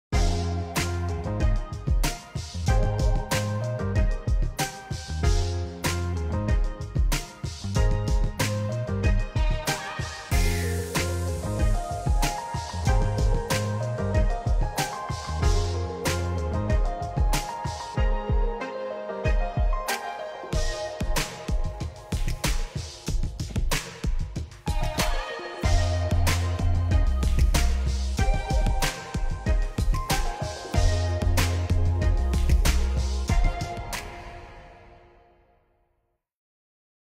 2020 Yamaha Grizzly EPS - Green
Check out the quick overview of the new green Yamaha Grizzly 700 with EPS.and black aluminum rims.